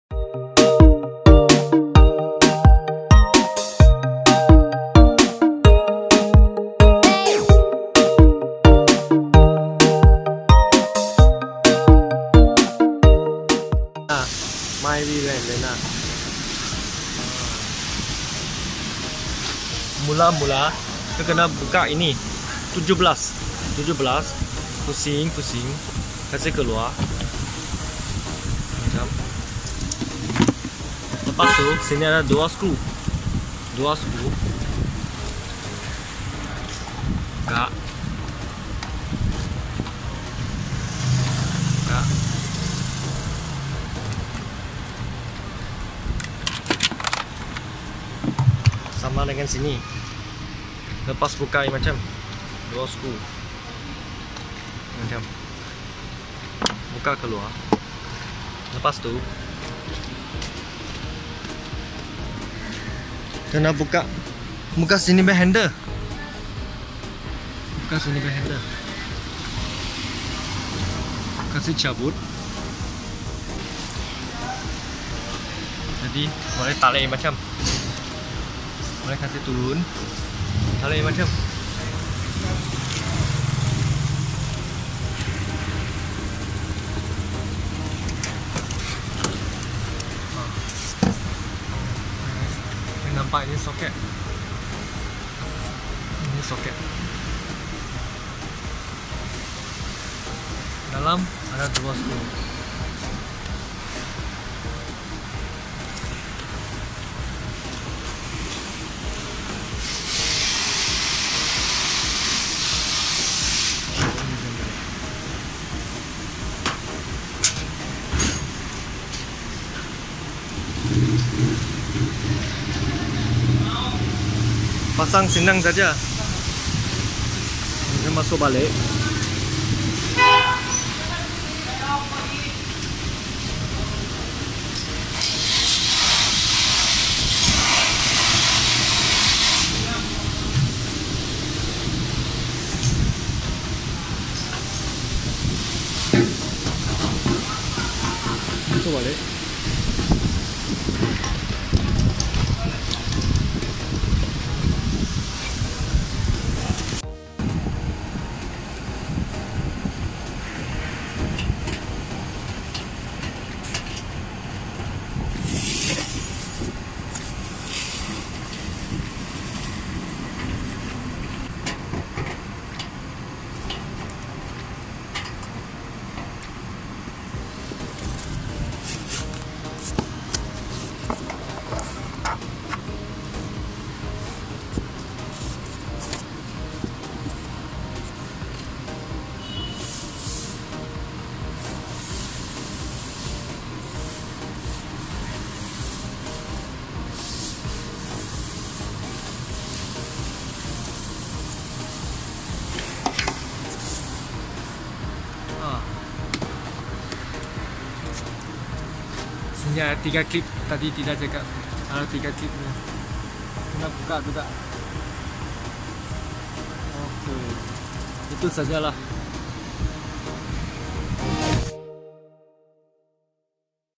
How to install Perodua Myvi Antenna Assy
This video shows how to change rear whole antenna unit.